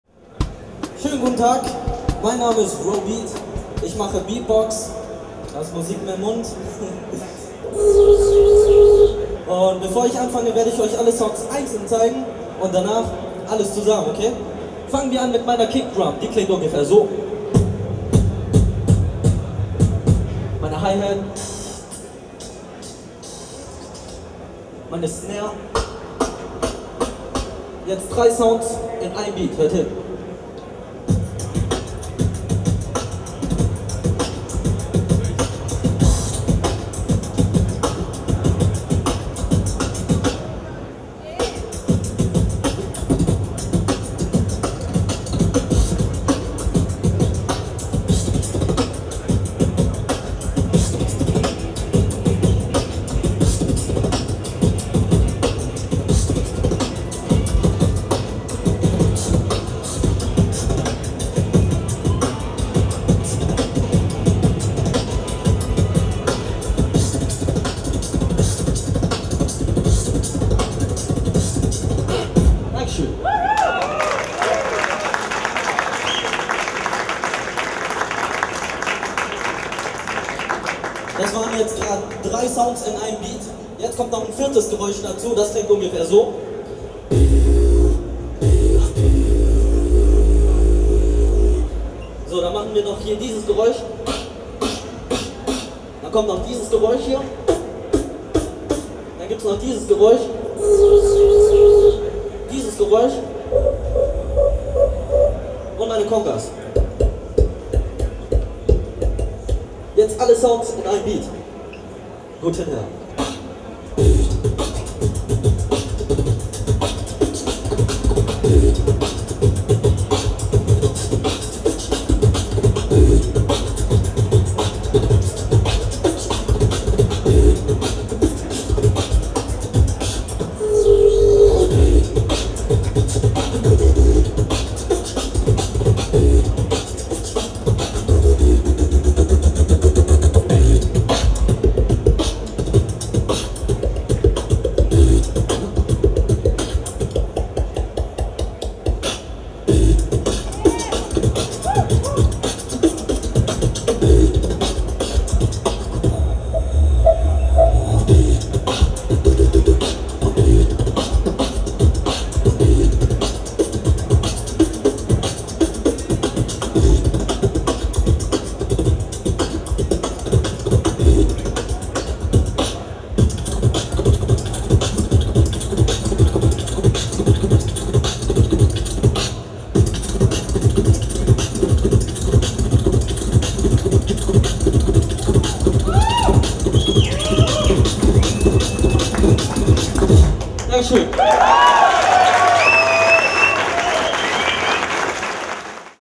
Teil 1: Robeat Live auf der Prolight + Sound 2009
Der Deutsche Vize-Meister im Beatboxen, Robeat, verschlägt den Zuschauern immer wieder die Sprache. Als Human Beatbox, also nur mit Mikrofon und Stimme ausgestattet, demonstriert er die hohe Kunst der Mundakrobatik --- Teil 1 des fulminanten Live-Auftritts am Stand von Shure auf der Prolight + Sound 2009.